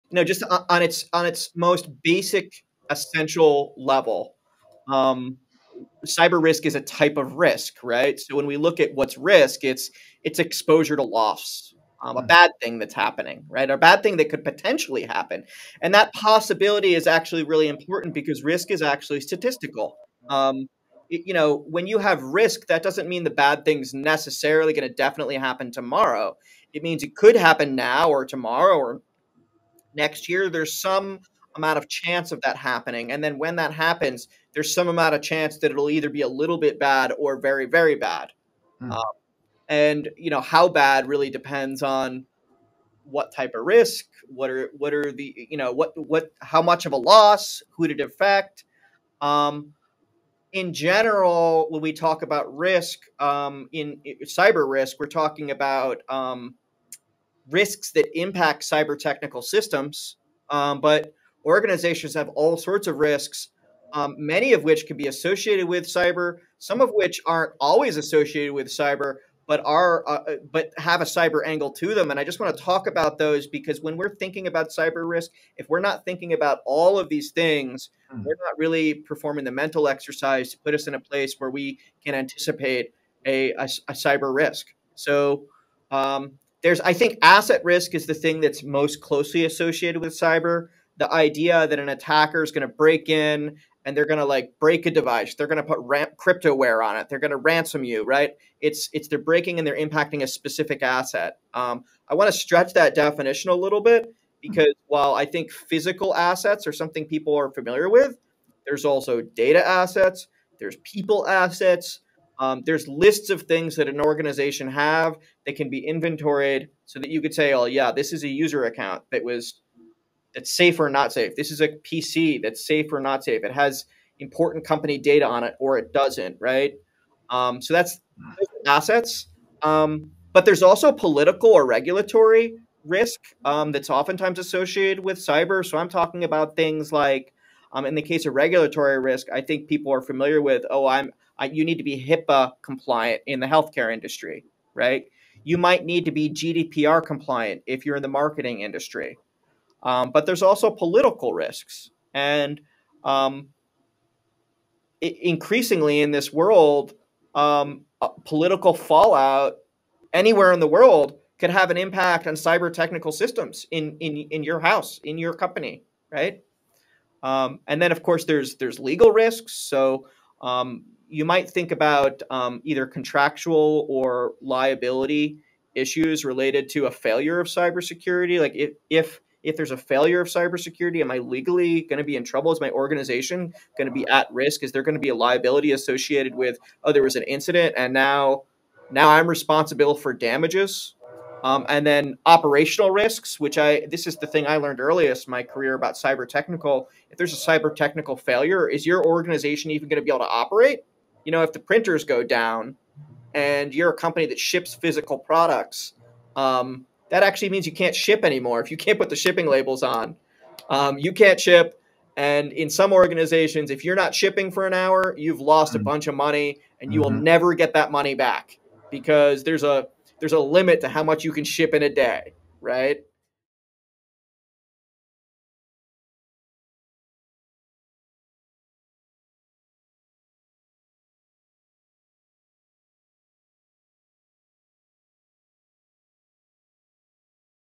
What is cyber risk and types of risk
Matthew Fisch from FortMesa explains what is cyber risk and types of risks.

Watch the whole episode about what is cyber risk and how to empower consumers by bridging the cybersecurity knowledge gap, providing essential insights for safer practices here: